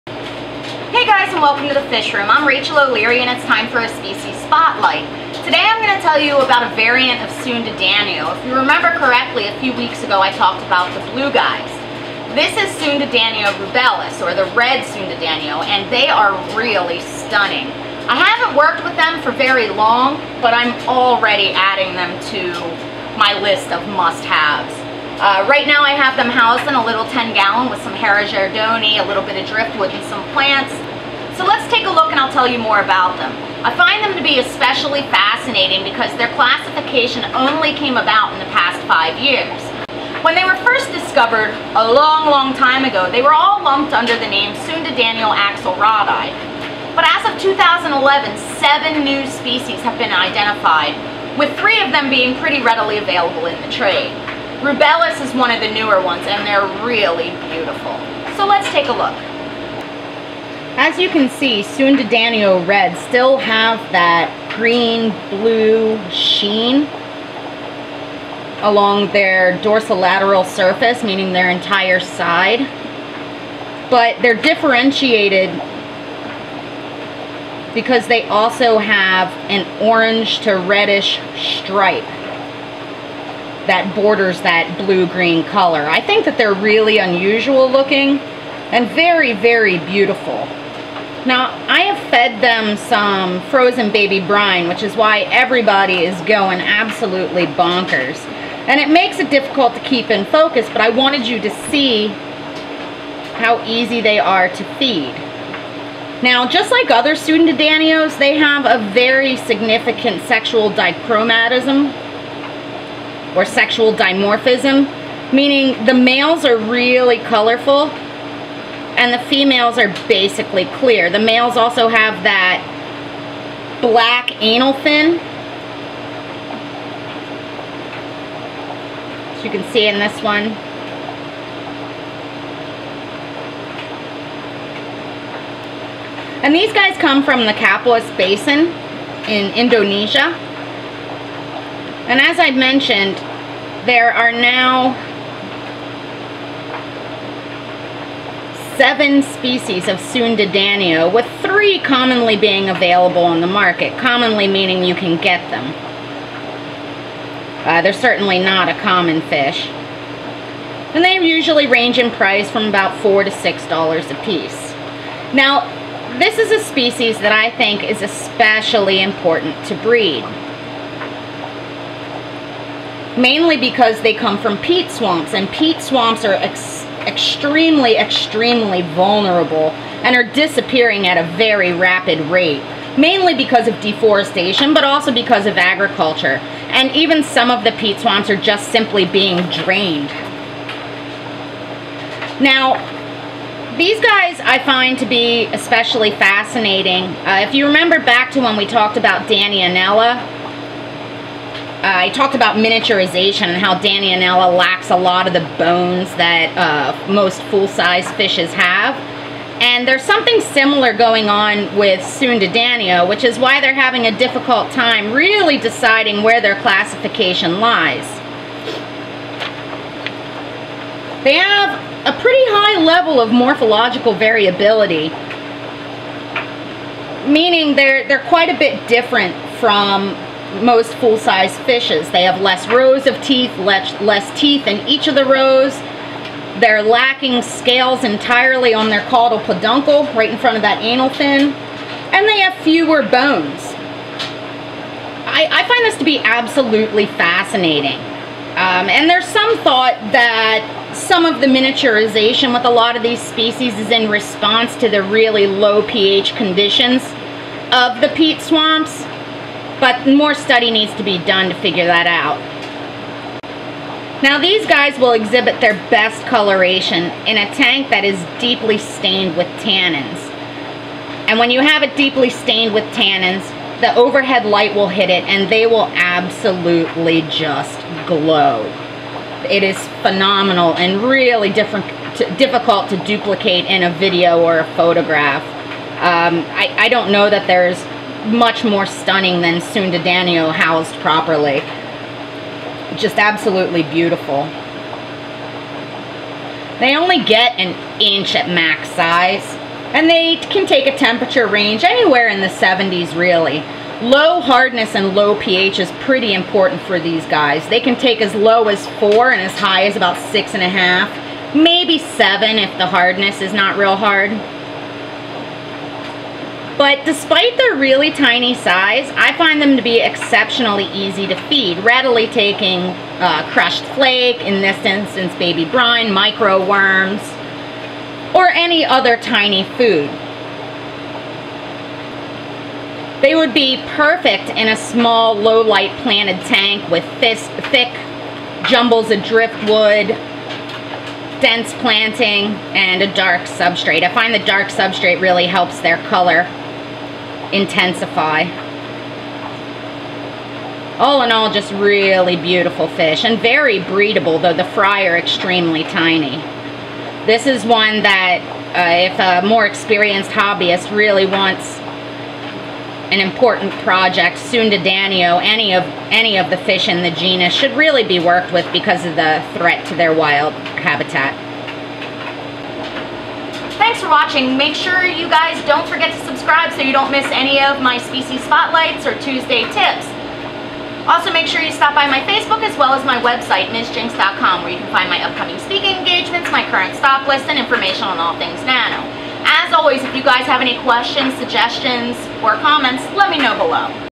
Sundadanio Red -Tiny and beautiful!
Tiny, irridescent green/blue with a red stripe- these littel stunners are an excellent project for the devoted hobbyist and quickly becoming a favorite of mine.
Sundadanio rubellus- Sundadanio red
Temp 72-79
Adult size .75"-1"
pH 4-7
Diet: small, meaty foods